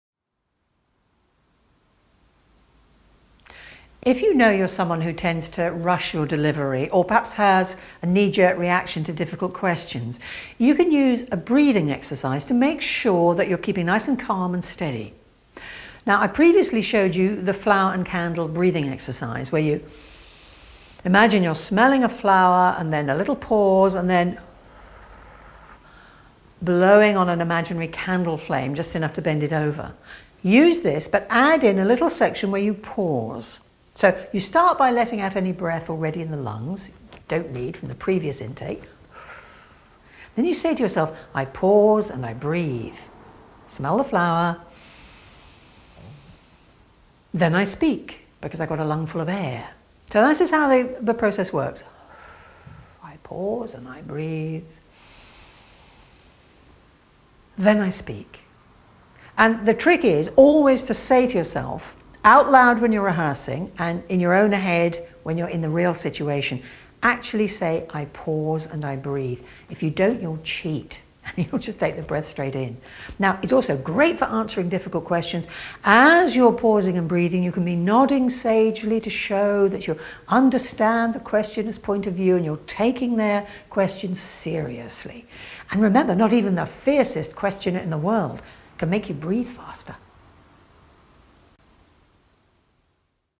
Pause and Breathe - to help you take your time
Many of us speak too quickly or answer difficult questions without giving our brains time to kick into gear!  Sometimes we lose focus and need ways to help us get back on track. Here Dilly shows a simple but very effective breathing exercise to help you take your time and/or regain focus.